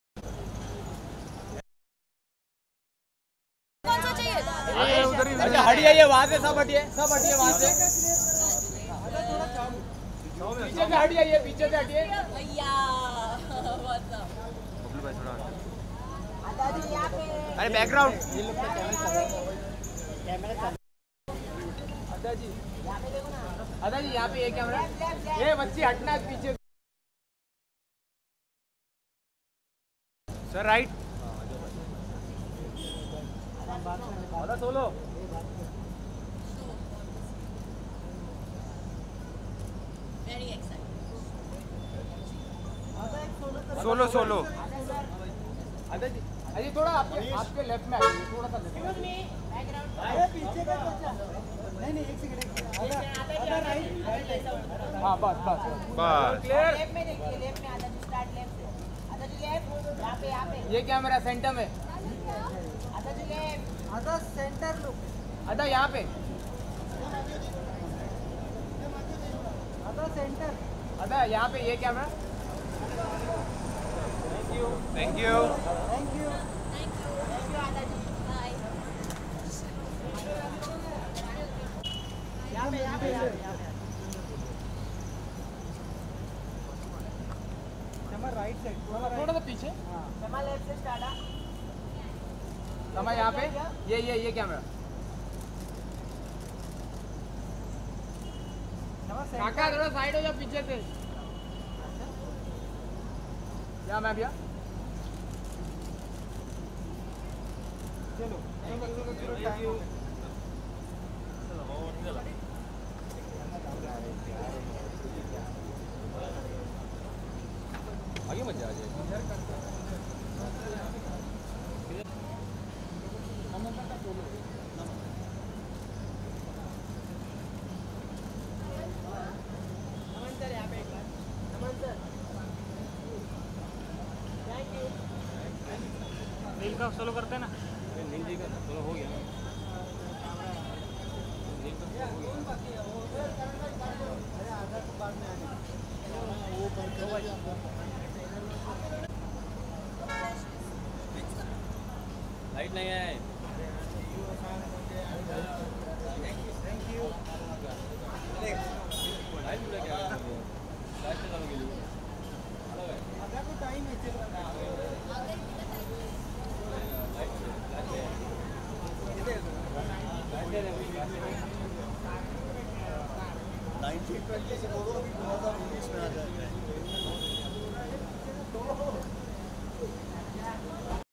Adah Sharma,Shama Sikander,Neil Nitin Mukesh At Trailer Launch Of Bypass
Adah Sharma,Shama Sikander,Neil Nitin Mukesh At Trailer Launch Of  Movie Bypass